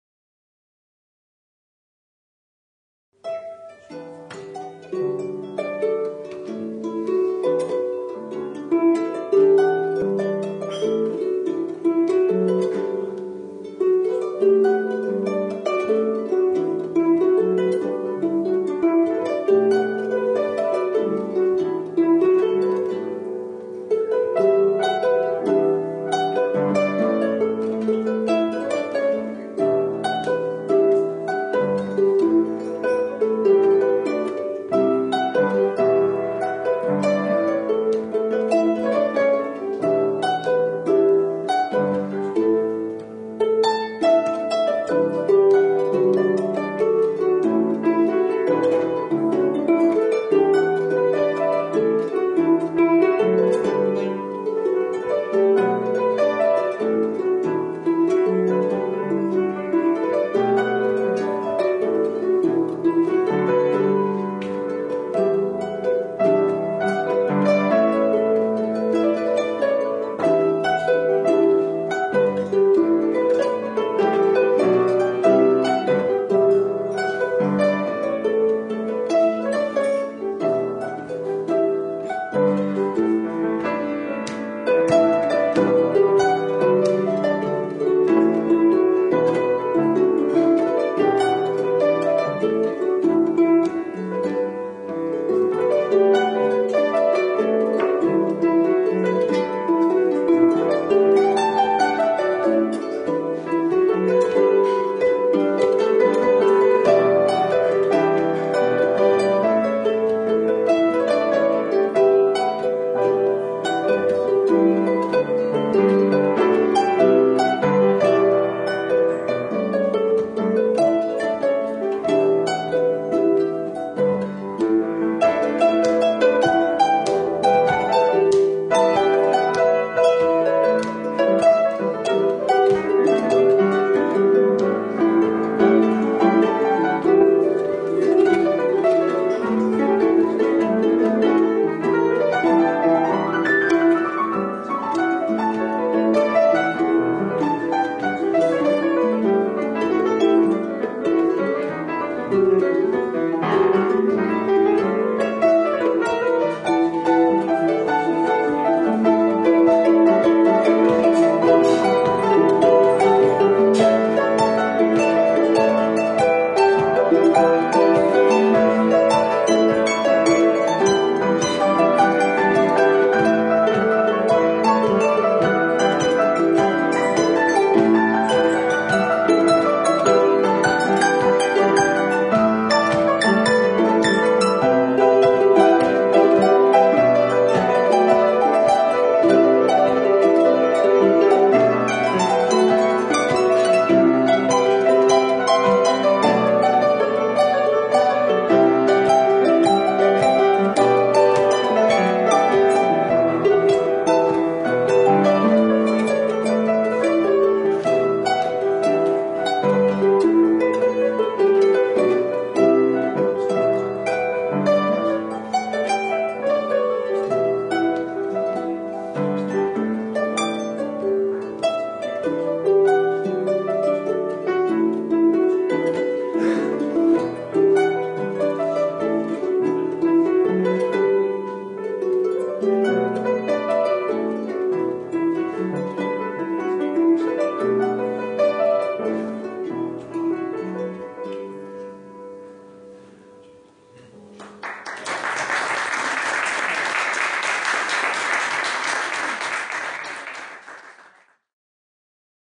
piano and harp duet live in Cafe Cossachok
Catriona McKay (harp) "Instrumentalist of the Year" at Scots Trad Music Awards 2007 and Phil Alexander (piano) perform in the Russian Cafe Cossachok 29th April 2007